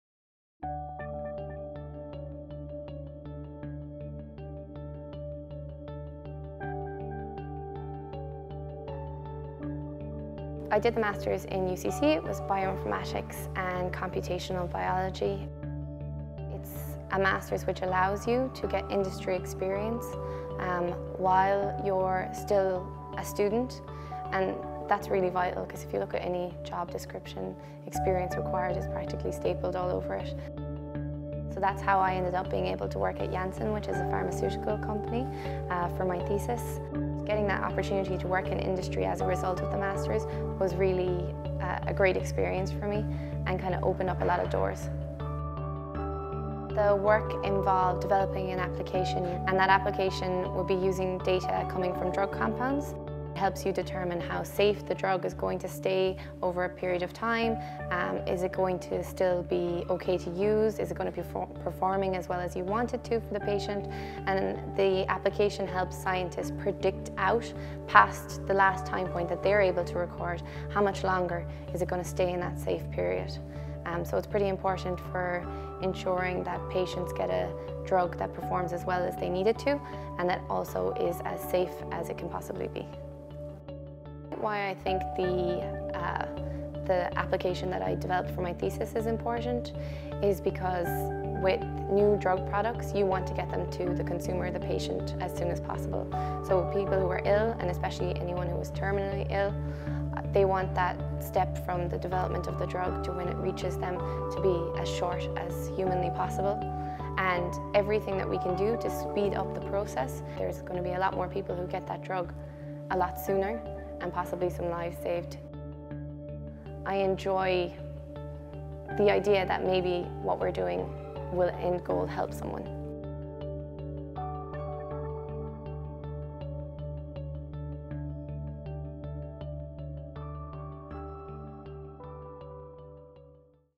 MSc Bioinformatics and Computational Biology at UCC
Rachel Fitzgerald talks about her journey from masters to PhD while studying Bioinformatics and Computational Biology at UCC.